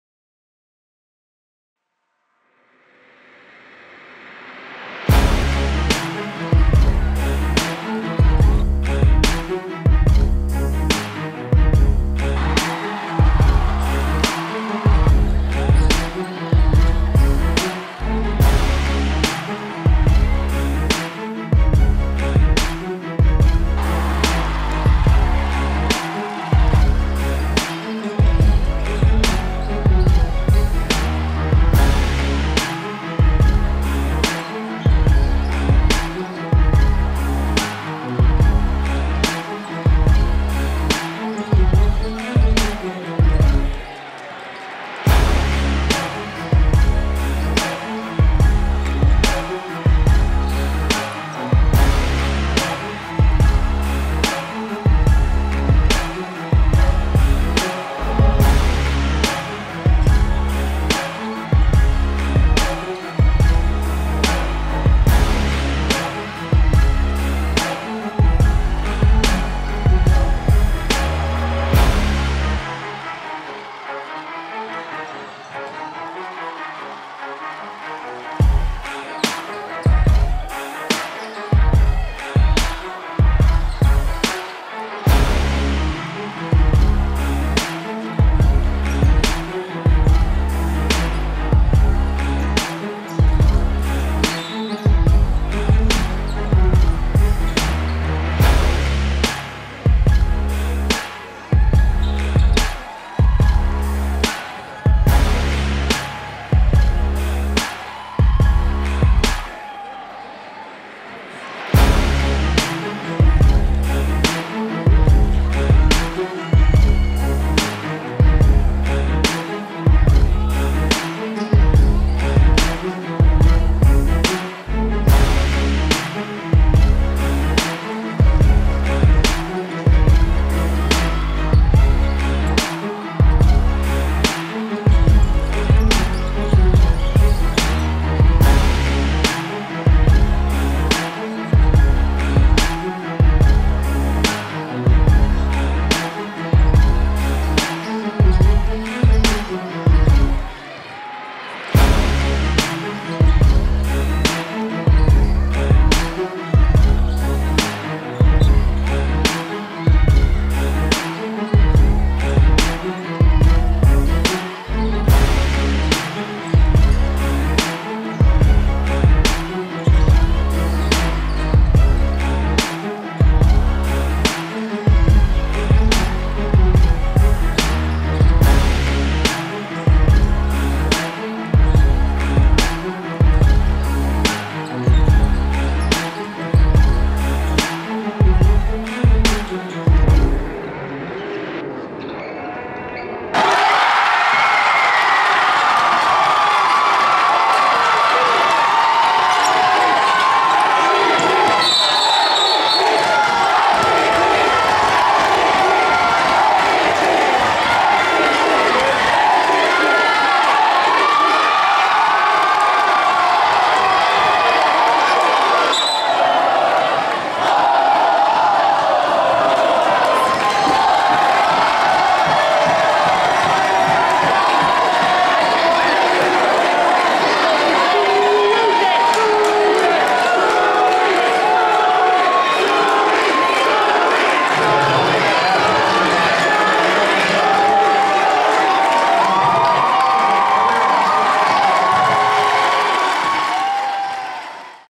Amherst Central High School - Jaylen Stewart 1,000 Point Milestone Game (Highlight Reel)
On February 28th 2020, Jaylen Stewart, of Amherst Central High School, achieved his very first 1,000 point milestone during the season's first playoff game against Williamsville South in the Buffalo Sectional.

Our Feature Length Film!